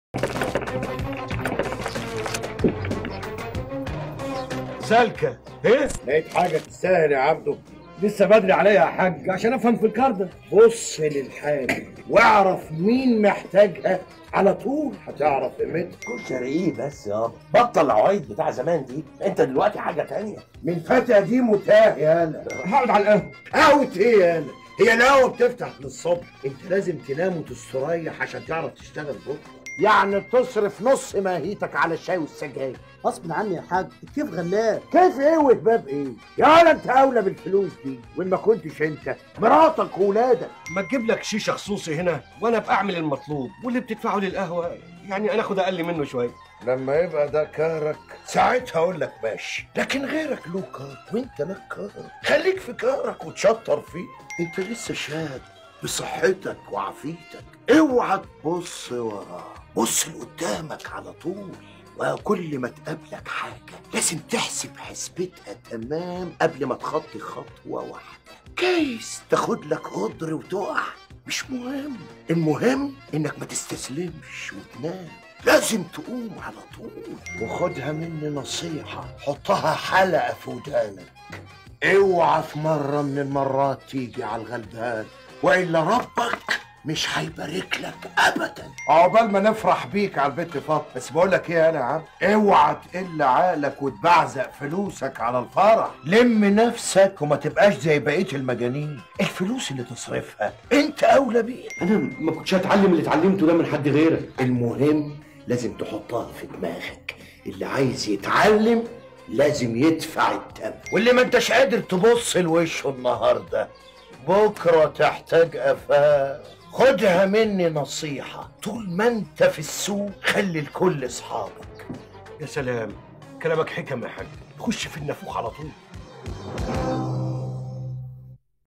10 قواعد أساسية للنجاح في الأعمال من المعلم إبراهيم سردينة
في هذا الفيديو، نقدم لكم 10 قواعد ذهبية للنجاح في عالم الأعمال من المعلم إبراهيم سردينة. هذه النصائح المميزة، المستمدة من سنوات من الخبرة في مجال الأعمال، ستساعدك على التخطيط بشكل استراتيجي، والتحلي بالصمود، وإدارة مواردك بذكاء، وبناء علاقات قوية. انضم إلينا لتتعلم كيف يمكن لهذه القواعد أن ترشدك نحو تحقيق النجاح والتميز في رحلتك المهنية.